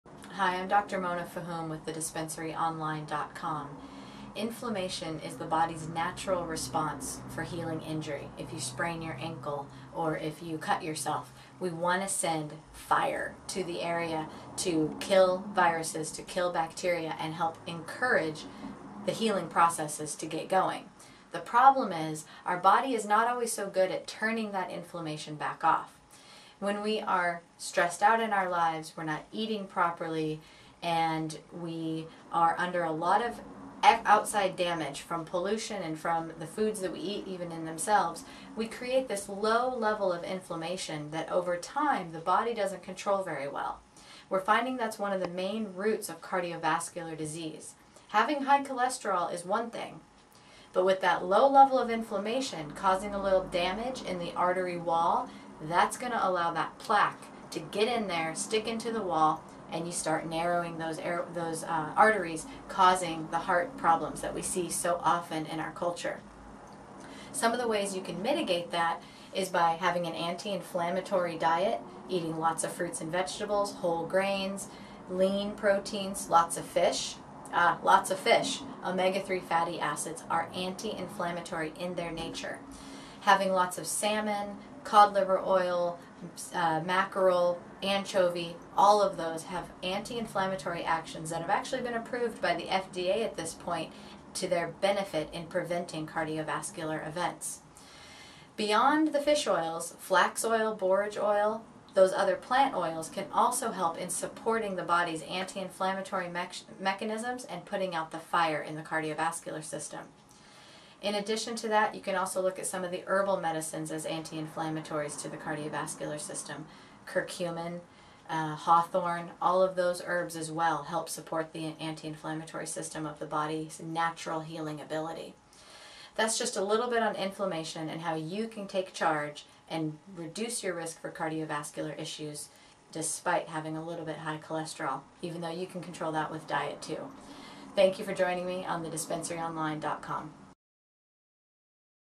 Natural Anti-Inflammatories Can Reduce Your Risk For Cardio Vascular Disease
Natural anti-inflammatories can help reduce your risk for cardiovascular disease.  These include flax oils, borage, circumen, hawthorne, and more.  Dr. Mona Fahoum of TheDispensaryOnline.com discusses natural anti-inflammatories and cardiovascular disease.